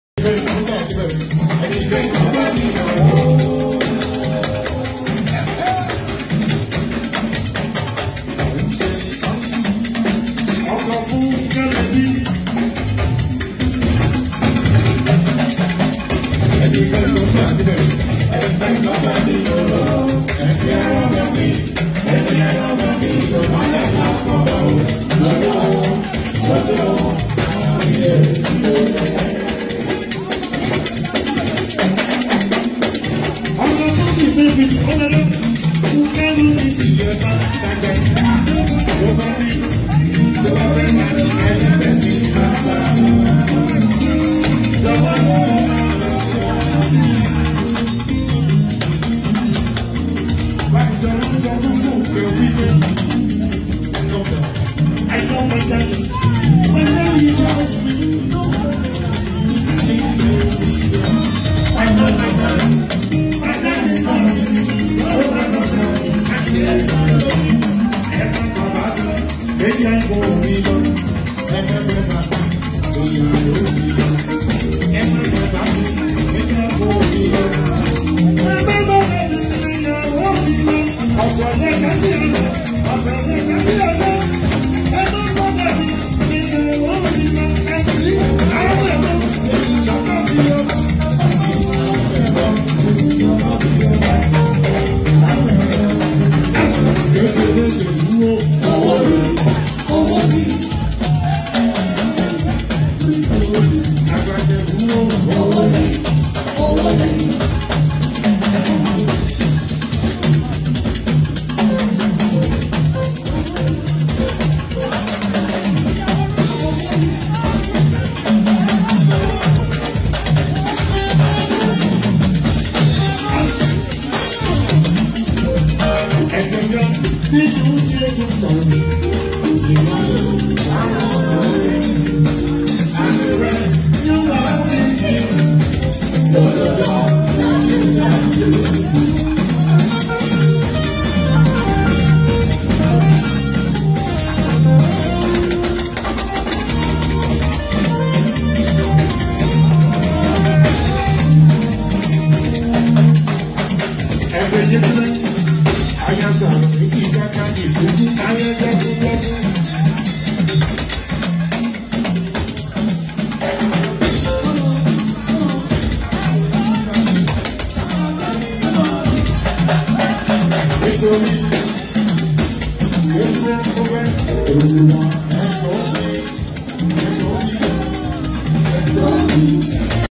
LS Entertainment Music Empire Gospel/Juju LIVE in Baltimore!
LS Entertainment Music Empire at Mrs. Folasade Sekiteri's golden jubilee! It was a fab time for us at this 50th birthday celebration!